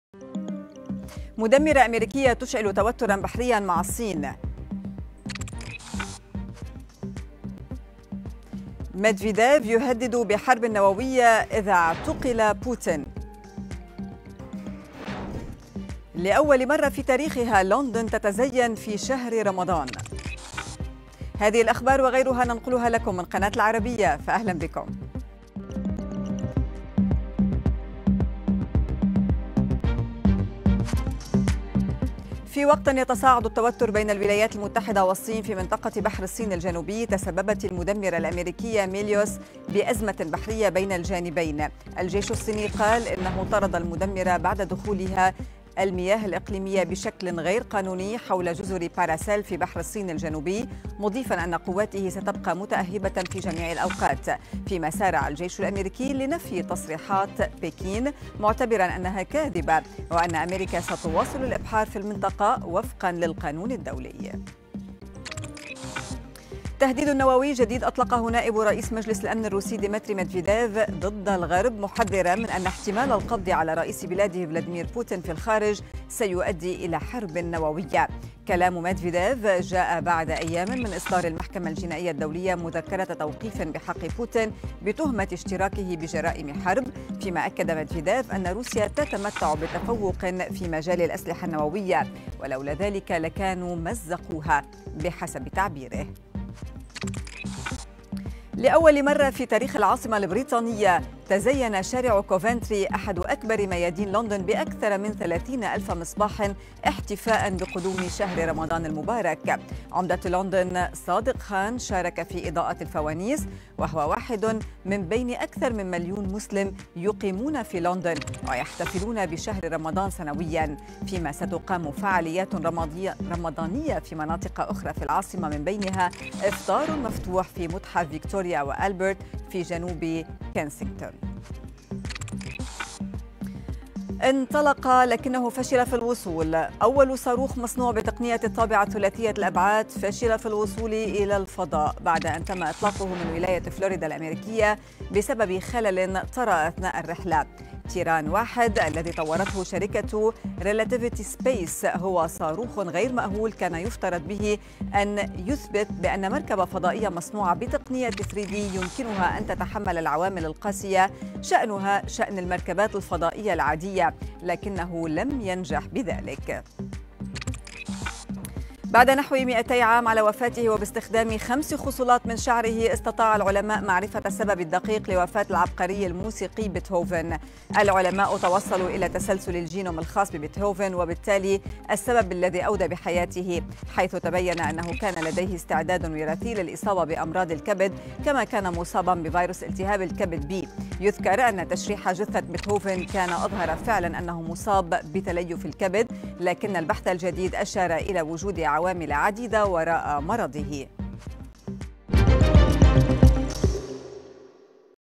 الأخبار في دقيقتين | ميدفيديف يهدد بحرب نووية إذا اعتقل بوتين
الأخبار في دقيقتين:
1️⃣ مدمرة أميركية تستفز الصين قرب سواحلها
2️⃣ ميدفيديف يهدد بحرب نووية إذا اعتقل
3️⃣ لأول مرة في تاريخها.. لندن تتزين في شهر